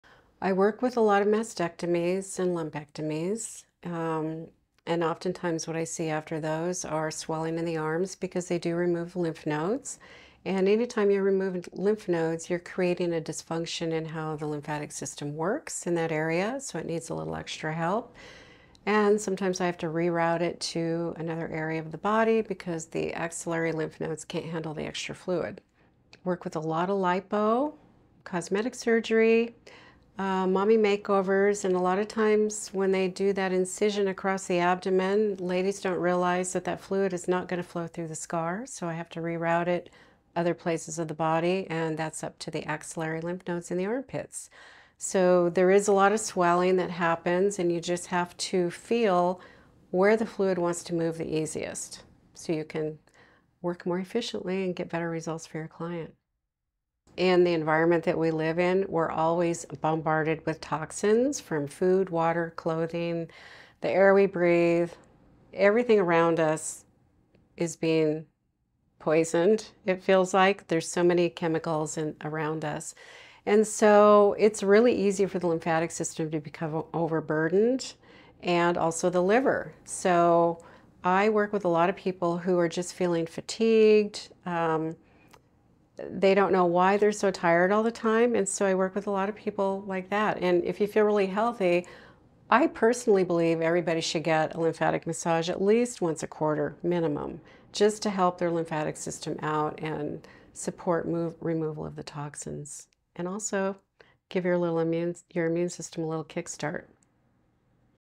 MLD Who I Work With
Lipedema vs. Lymphedema: What's the Difference?

Lipedema and lymphedema are frequently mistaken due to their similar names and overlapping symptoms. Nonetheless, they are separate and intricate conditions characterized by distinct causes, symptoms, and treatment strategies.